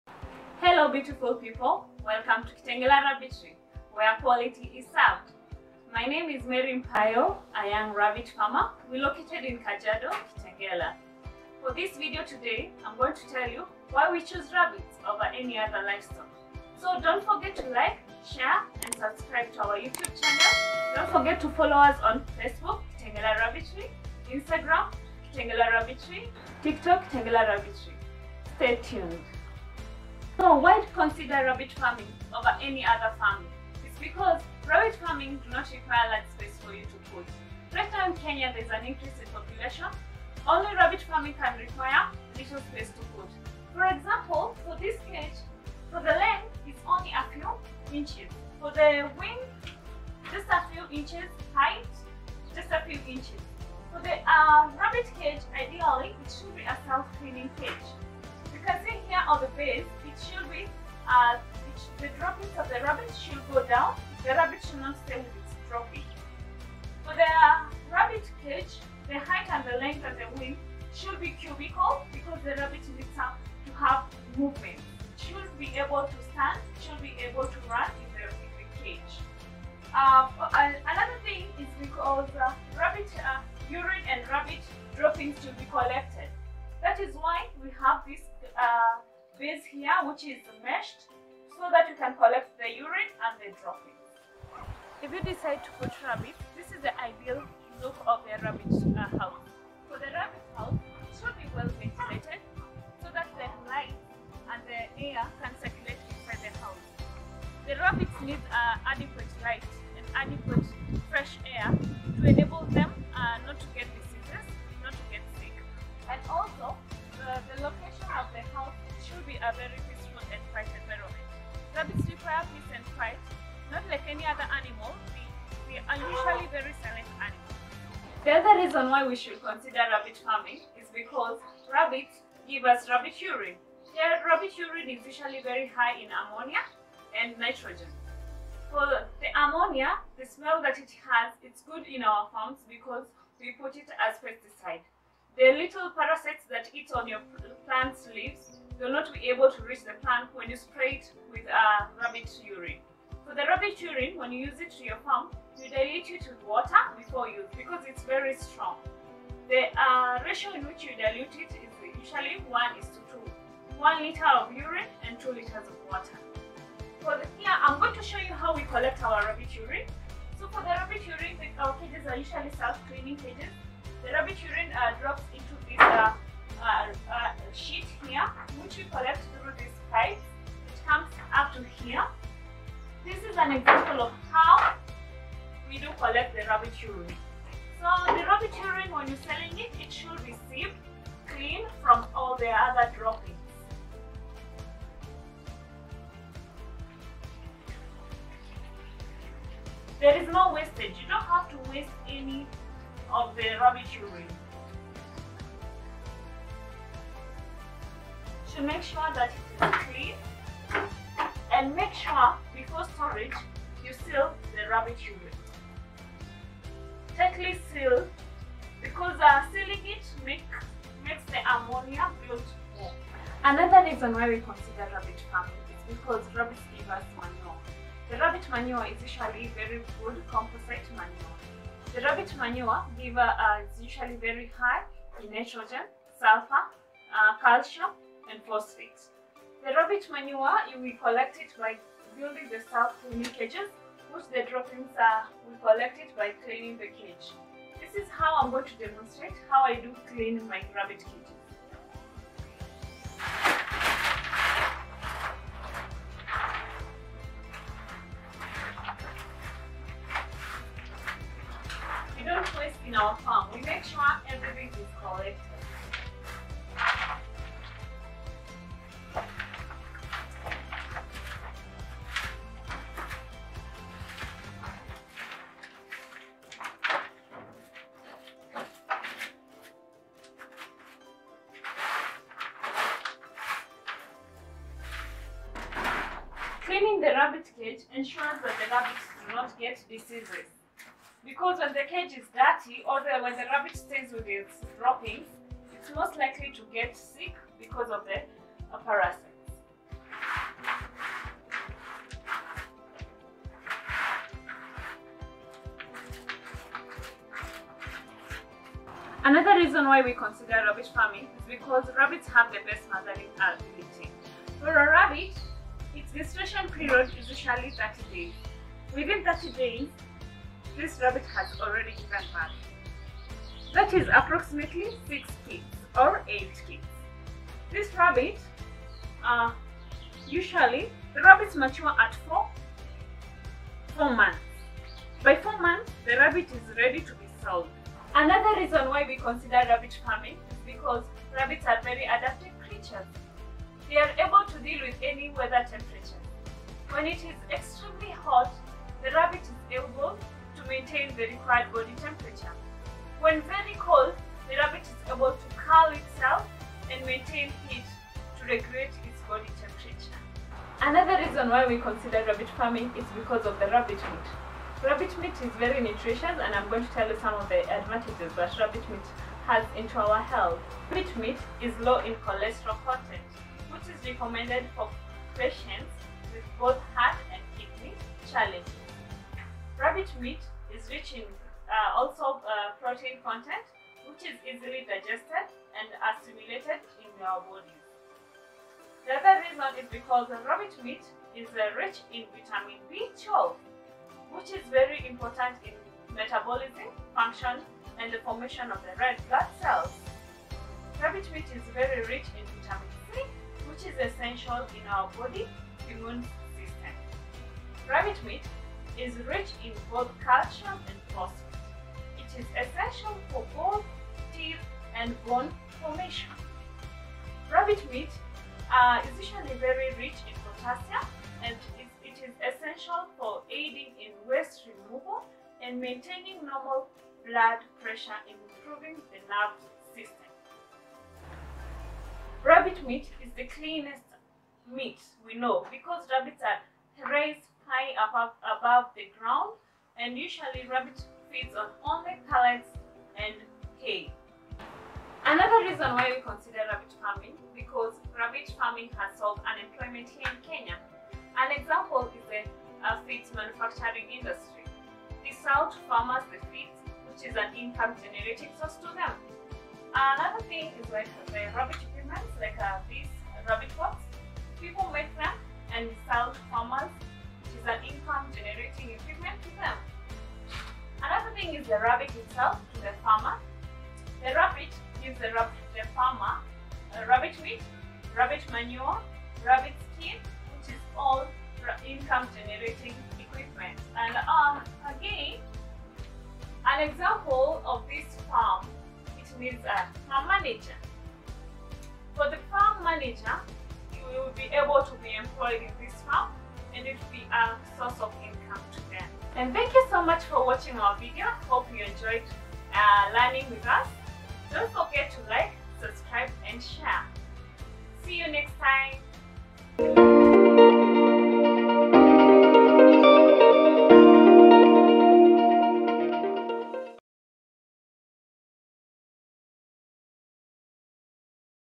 WHY RABBIT FARMING IS IDEAL IN 2023? (RABBIT QUEEN 👑.)NO WASTE IN RABBIT FARMING
Kitengela rabbitry is a breeder based farm in Kajiado County in kitengela Town located along Namanga road 1.8km from Kitengela mall.We deal with Rabbit Breeders,Rabbit Meat, Rabbit Manure,Rabbit Urine,Rabbit fur and also offer training and mentorship program.